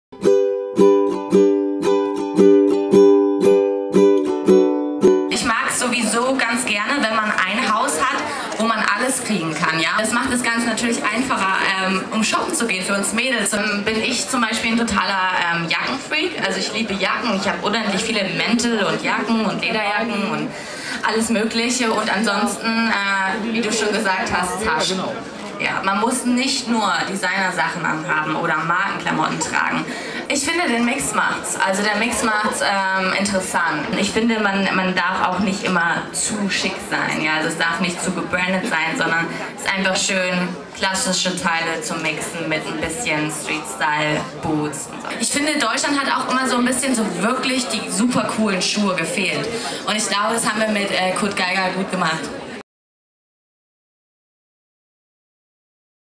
BEVOGUED TV: KARSTADT FASHION WEEKS MIT LENA GERCKE
Germany's Next Topmodel Gewinnerin Lena Gercke im Fashion Talk bei den Karstadt Fashion Weeks!
Germany's Next Topmodel winner Lena Gercke talking about fashion during the Karstadt Fashion Weeks.